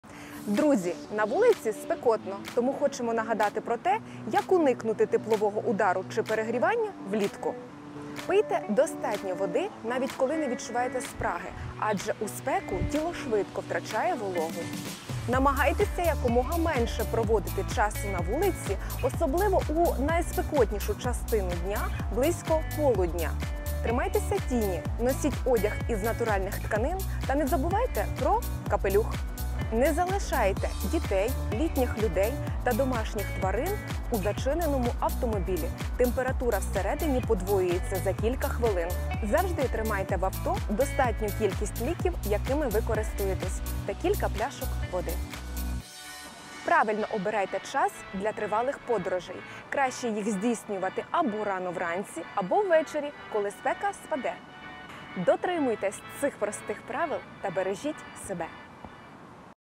Нагадуємо про те, як уникнути теплового удару чи перегрівання влітку
Друзі, за прогнозом Укргідрометцентру, кінець тижня буде спекотним

пийте достатньо води, навіть, коли не відчуваєте спраги. Адже у спеку тіло швидко втрачає вологу;

намагайтеся якомога менше проводити часу на вулиці у найспекотнішу частину дня — близько полудня. Якщо так сталося — тримайтеся тіні, носіть одяг із натуральних тканин та не забувайте про капелюх;

не залишайте дітей, літніх людей або домашніх тварин в зачинених автомобілях. Температура всередині під час спеки подвоюється за кілька хвилин;

завжди тримайте в авто необхідний запас ліків, якими ви зазвичай користуєтесь та кілька пляшок води. Краще використовуйте пляшки зі скла чи металу, оскільки під час нагрівання пластику у воду потрапляють шкідливі речовини.

правильно обирайте час для тривалих поїздок — найкраще їх здійснювати або рано-вранці, або ввечері, коли спека спаде.

Дбайте про свою безпеку.